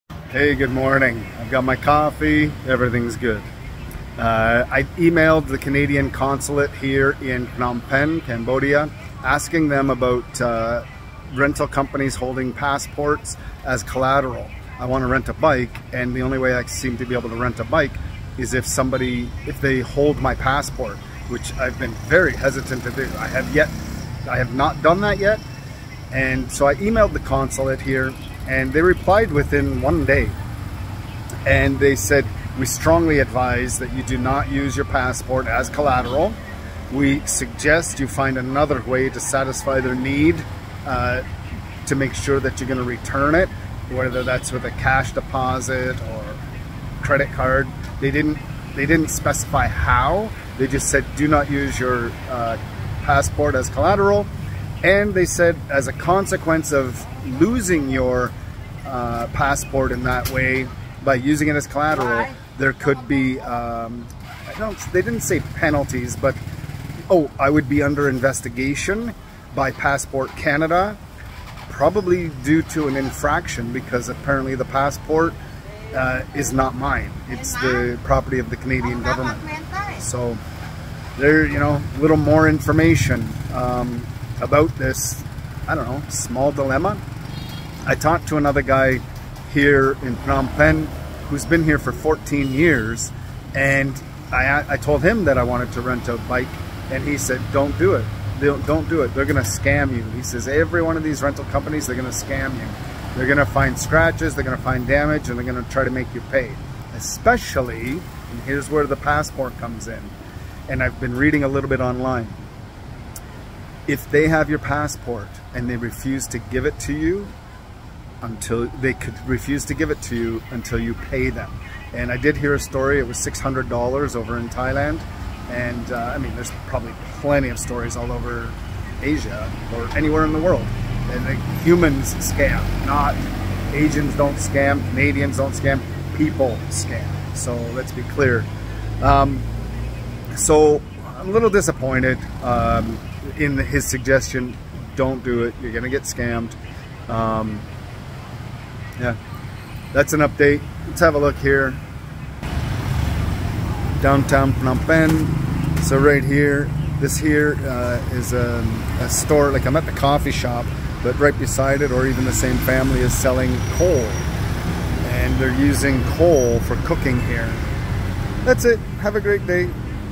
Canadian Consolate says, "No".
I hope the following helps you make an informed decision.

Thank you for contacting the Office of the Embassy of Canada in Phnom Penh.

We strongly advise against using your Canadian passport as collateral for rental. If your passport is inaccessible or stolen because of such a situation, you may be subject to investigation by Passport Canada and may receive limited passport services.

We suggest that you speak to the rental shop owner (to see) if any other items can be accepted.